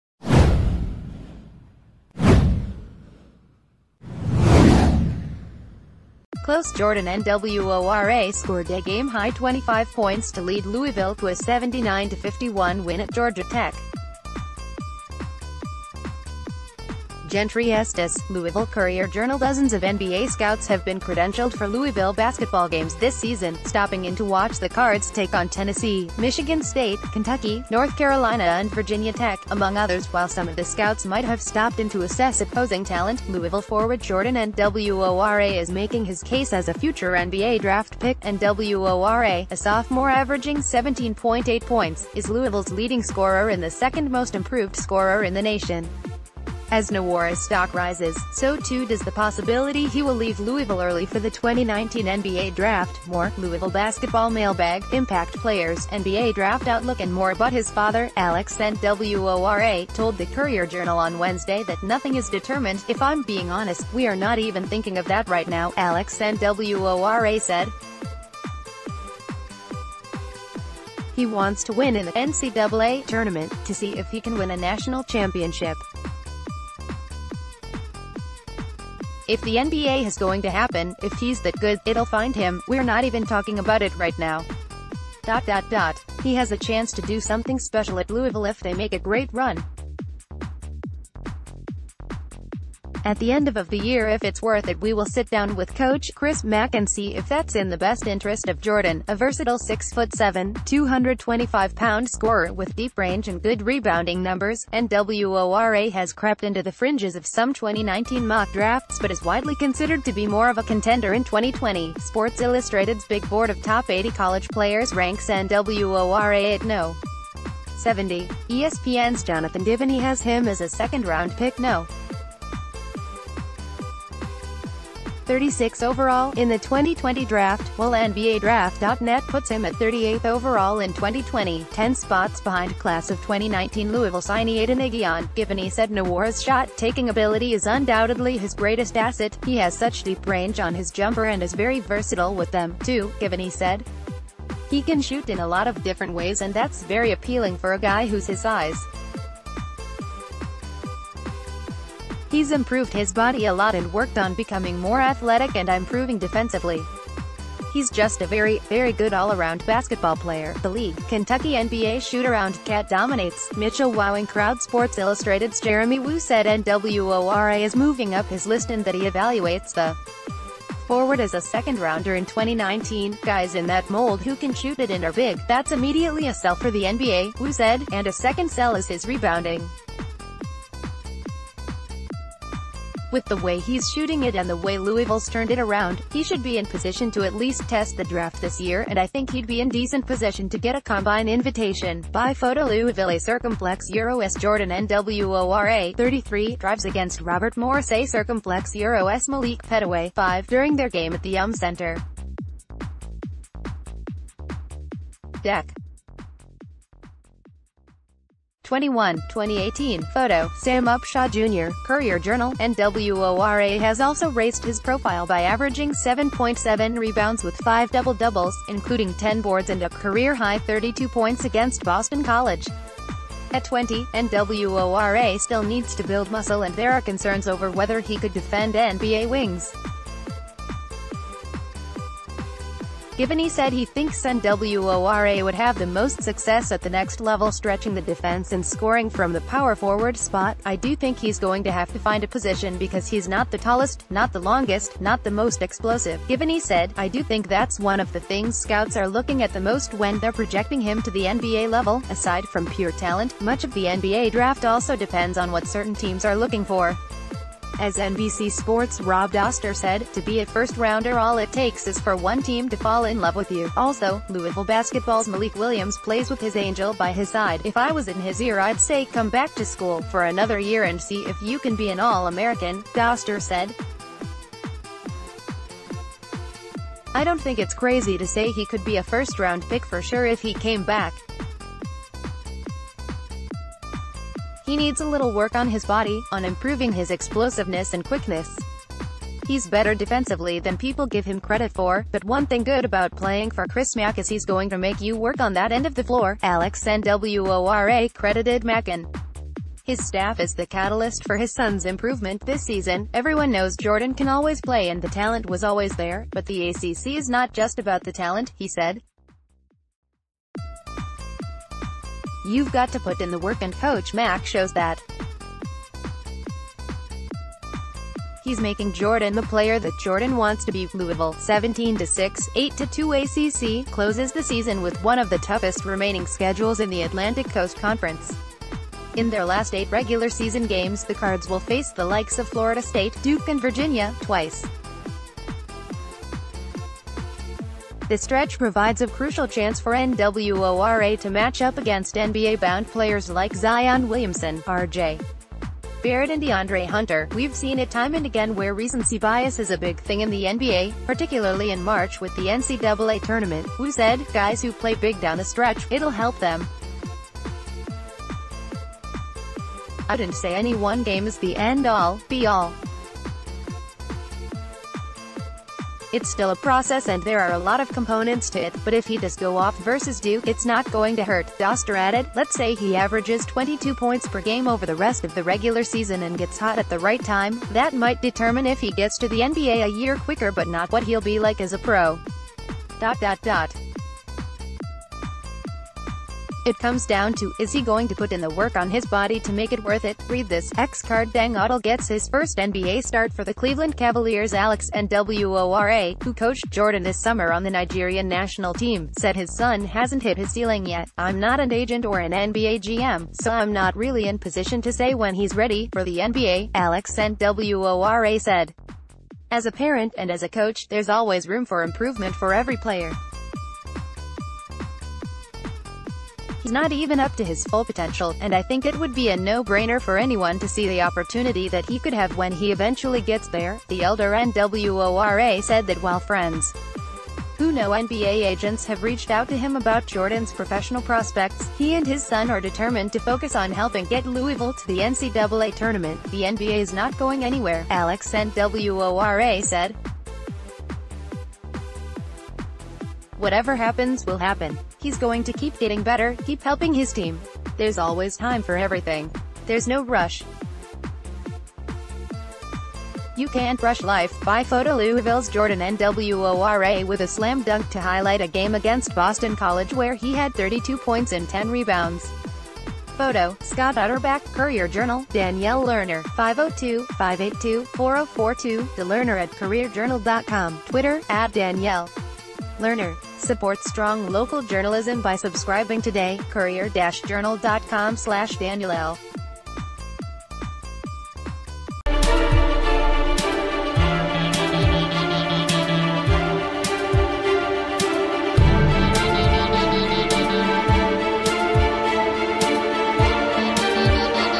Jordan Nwora in 'no rush' to leave Louisville for NBA, his dad says
CLOSE Jordan Nwora scored a game-high 25 points to lead Louisville to a 79-51 win at Georgia Tech. Gentry Estes, Louisville Courier JournalDozens of NBA scouts have been credentialed for Louisville basketball games this season, stopping in to watch the Cards take on Tennessee, Michigan State, Kentucky, North Carolina and Virginia Tech, among others. While some of the scouts might have stopped in to assess opposing talent, Louisville forward Jordan Nwora is making his case as a future NBA draft p...